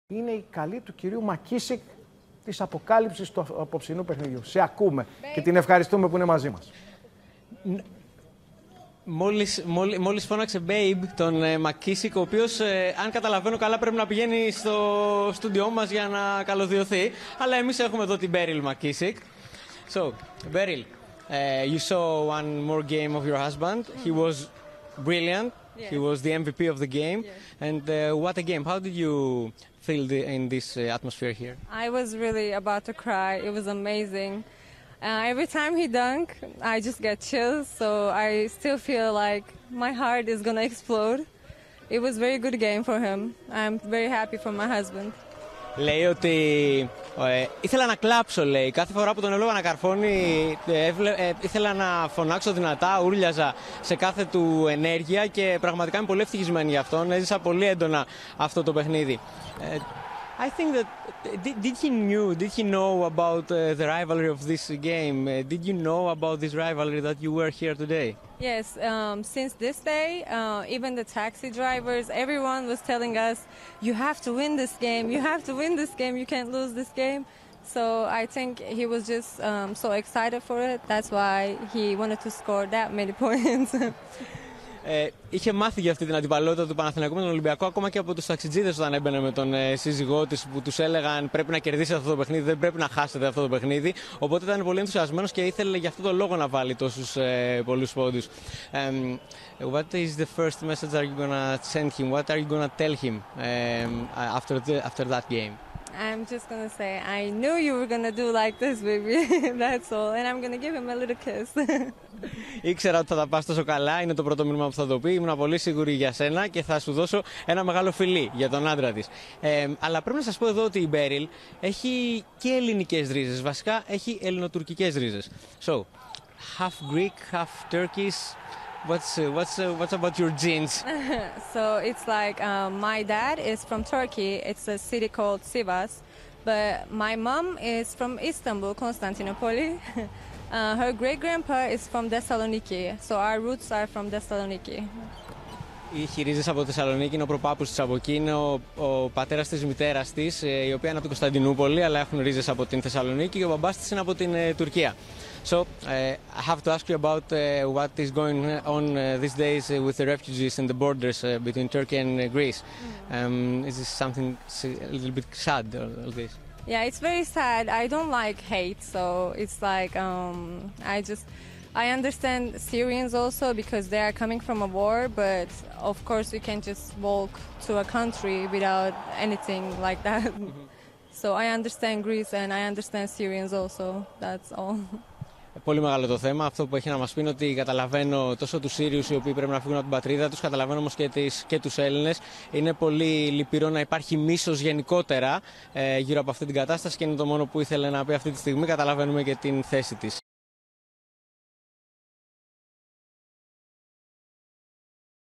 Σύντροφος ΜακKίσικ Ήμουν έτοιμη να κλάψω βλέποντας τον"
Η σύντροφος του Σακίλ Μακίσικ, Μπέριλ, δήλωσε συγκινημένη για την απόδοση του συντρόφου της!

ΜΠΑΣΚΕΤ  Euroleague